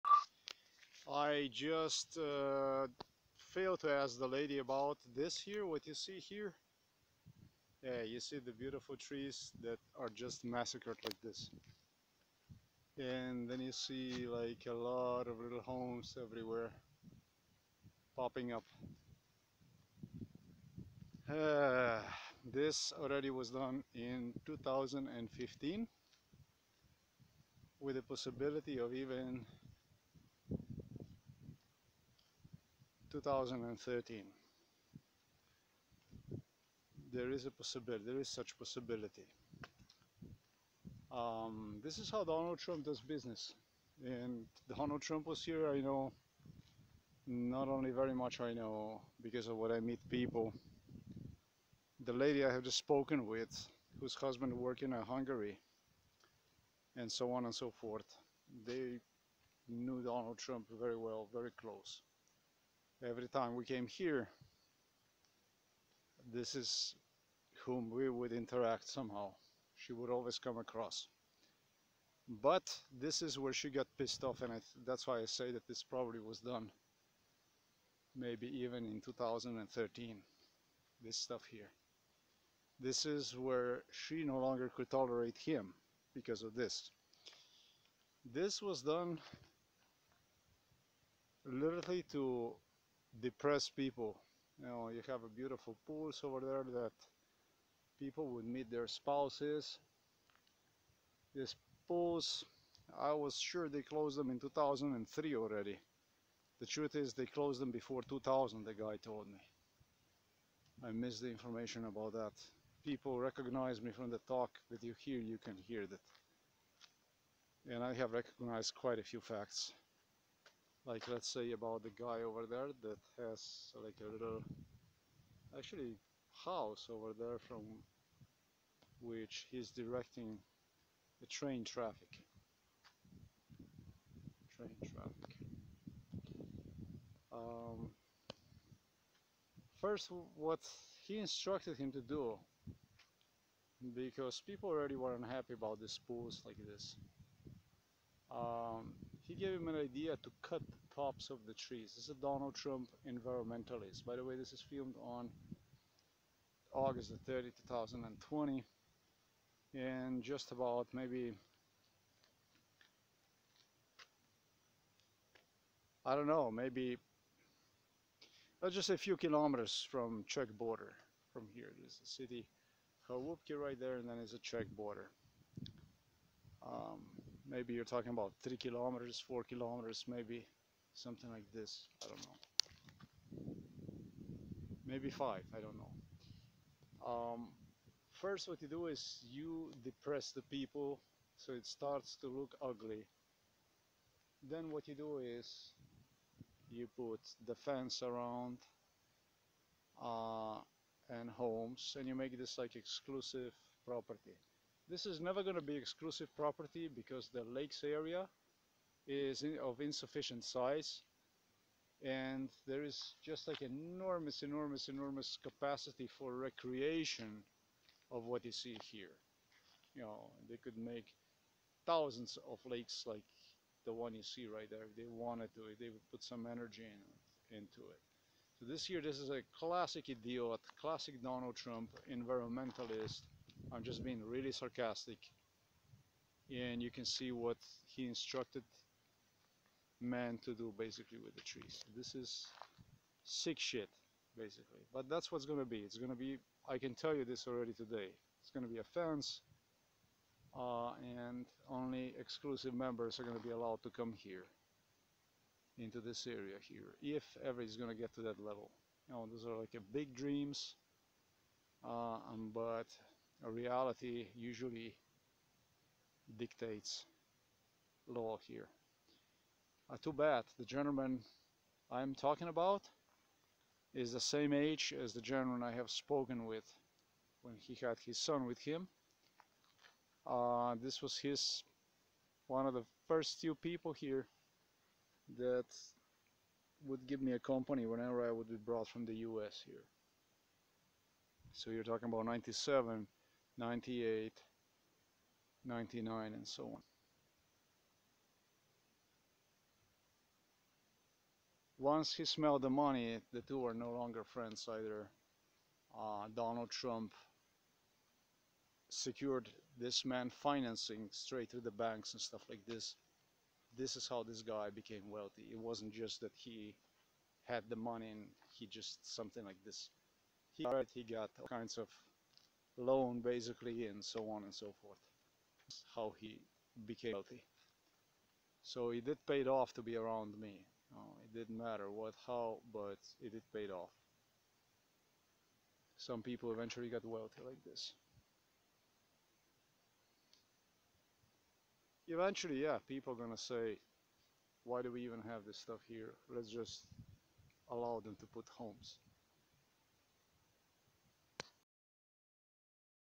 How dreams of Polish Olza Chałupki area locals were turned into tears via Donald Trump real estate a
How dreams of Polish Olza Chałupki area locals were turned into tears via Donald Trump real estate assistance I present Holiday Resort OLZA Ośrodek Wypoczynkowy OLZA